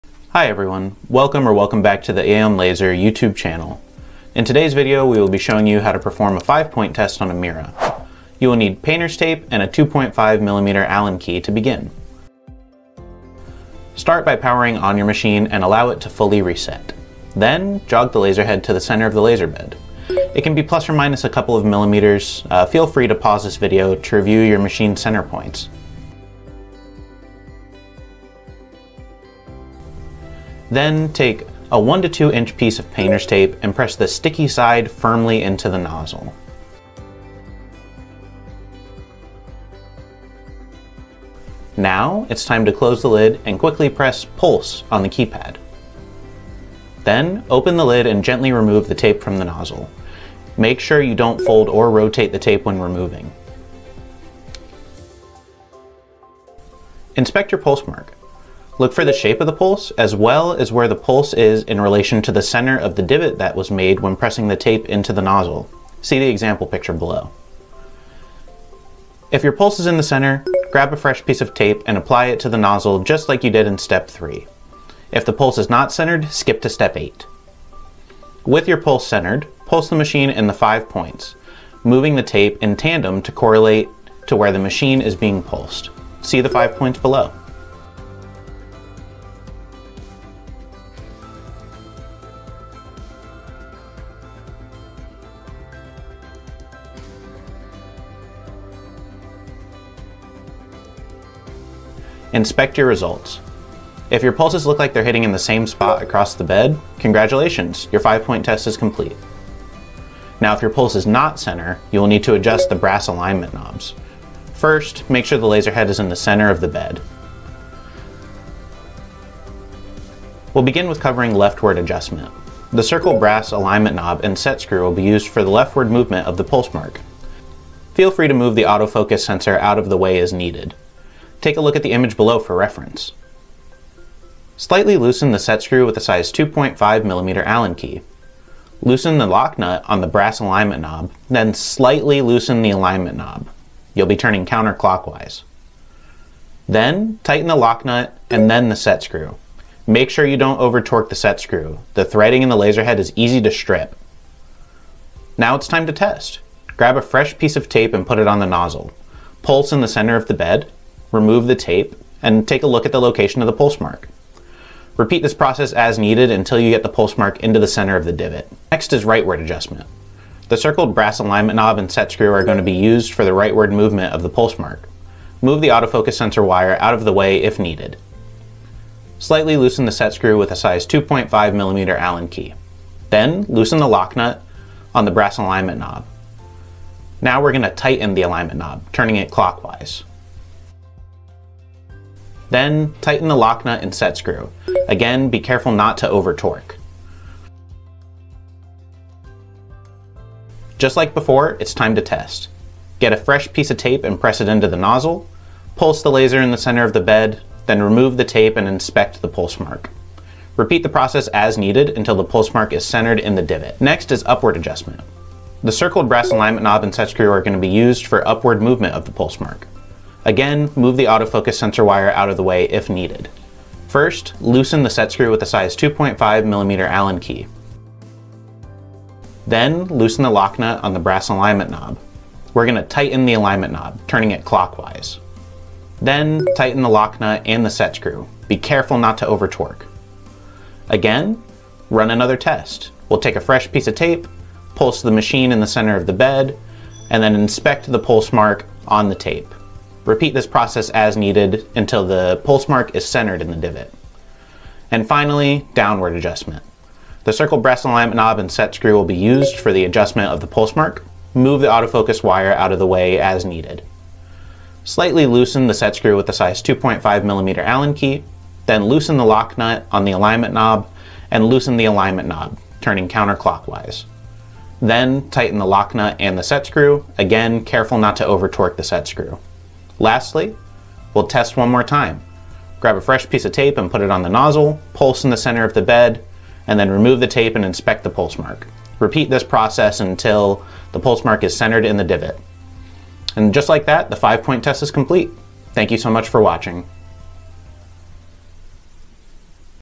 How to Perform a 5 Point Test
Learn How to Perform a 5 Point Test on your Aeon laser.